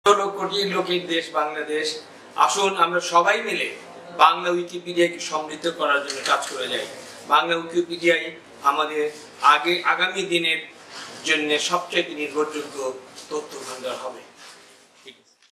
বাংলা উইকিপিডিয়া সমৃদ্ধ করুন - ফয়জুল লতিফ চৌধুরী
বাংলা উইকিপিডিয়া সমৃদ্ধ করুন।
- ফয়জুল লতিফ চৌধুরী, মহাপরিচালক (আগস্ট ২০১৪ - জুন ২০১৮), বাংলাদেশ জাতীয় জাদুঘর